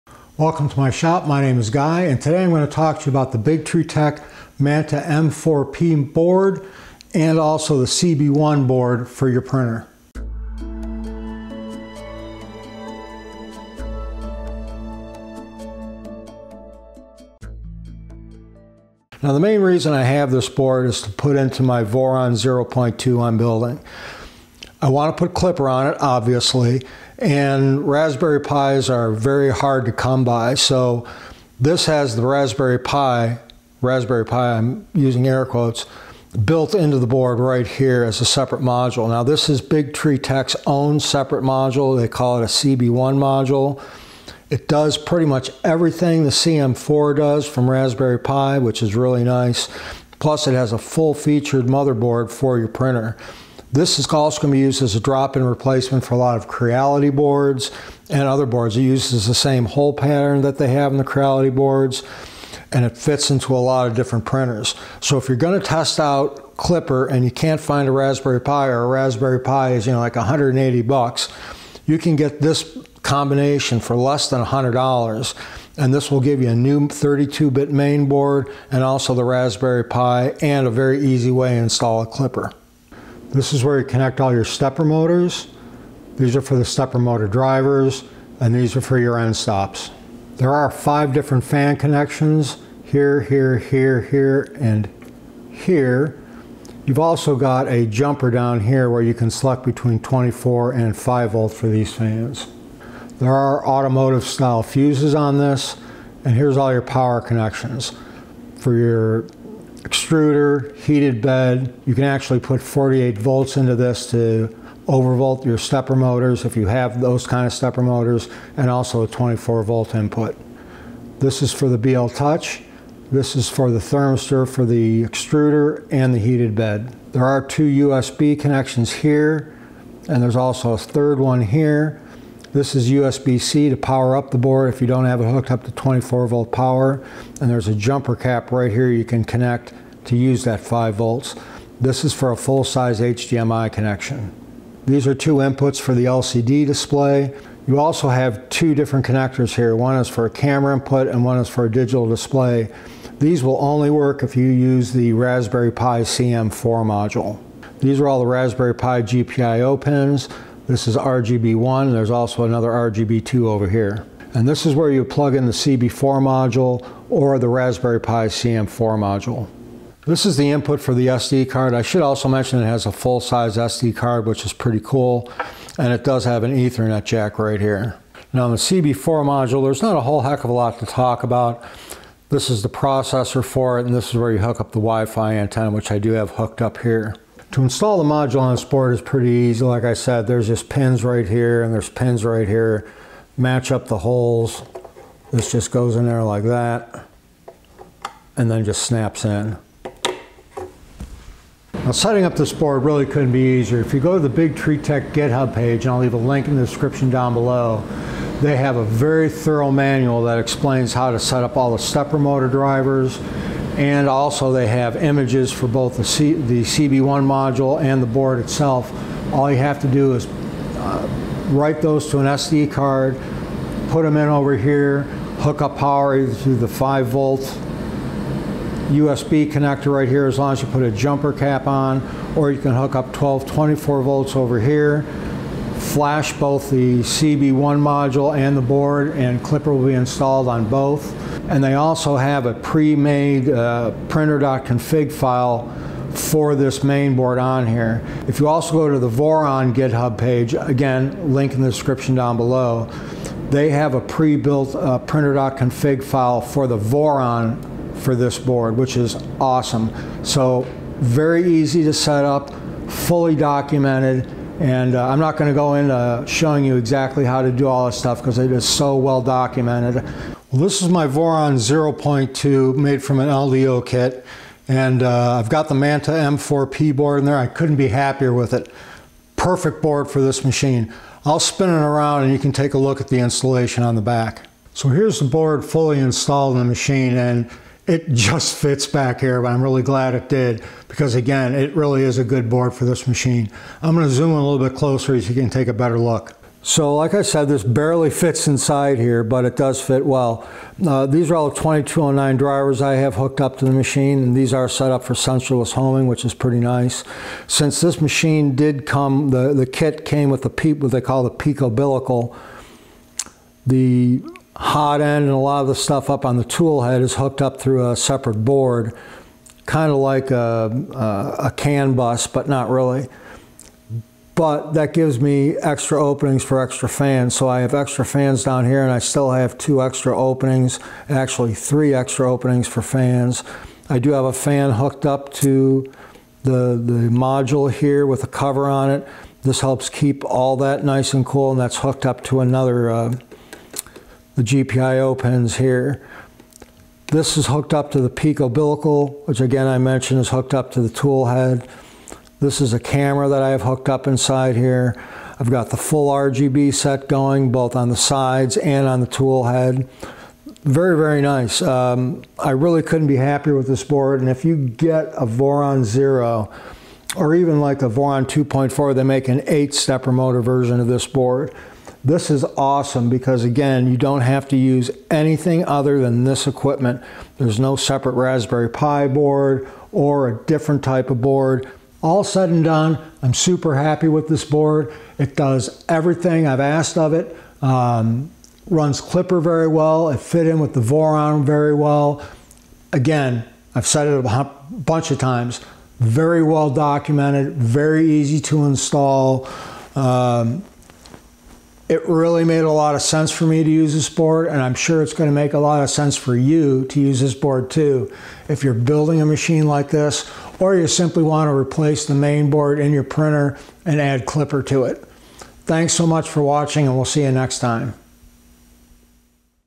Big Tree Tech Manta M4P & CB1 Review
A short review of the excellent BTT Manta M4P and CB1 that I installed in a Voron V0.2 3D Printer.